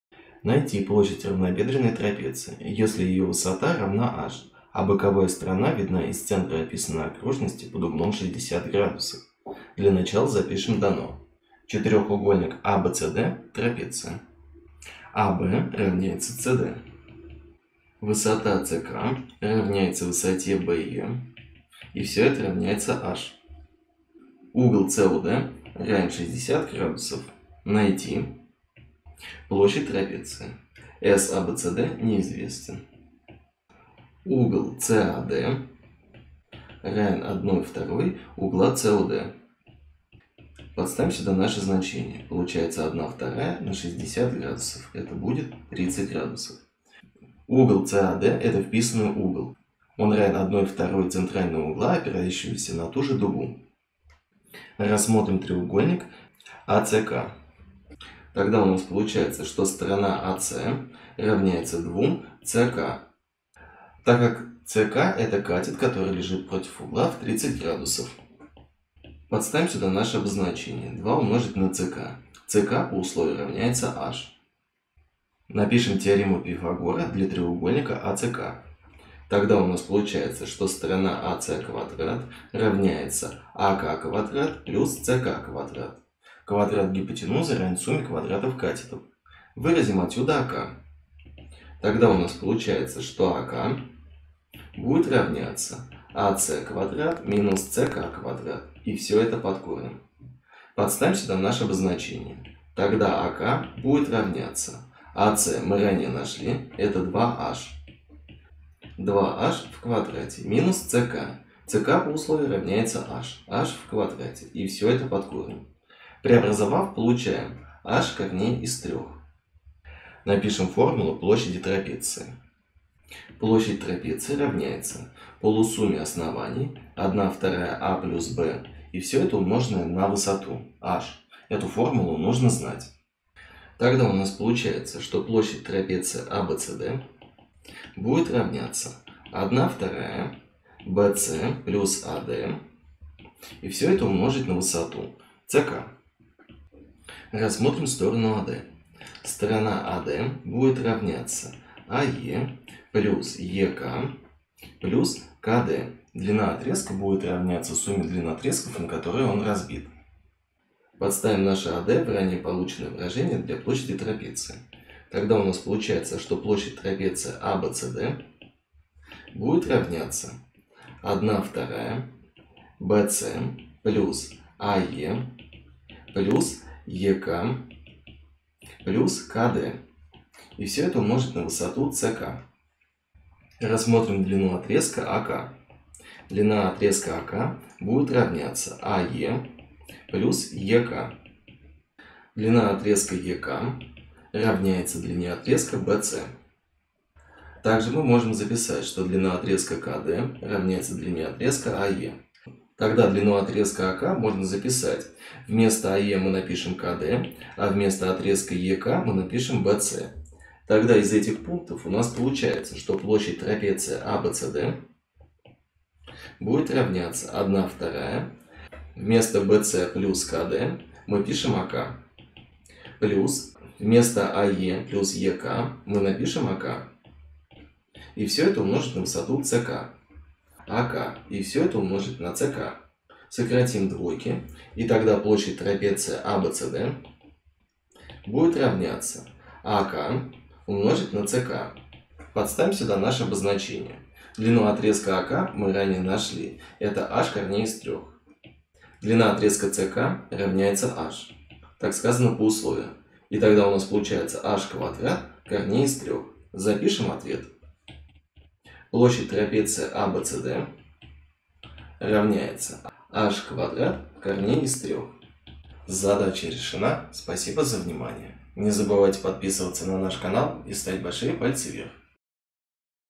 Геометрия Найти площадь равнобедренной трапеции, если ее высота равна h, а боковая сторона видна
Геометрия Найти площадь равнобедренной трапеции, если ее высота равна h, а боковая сторона видна из центра описанной окружности под углом 60.

Математика Модуль Геометрия подробное решение
Сканави 10,148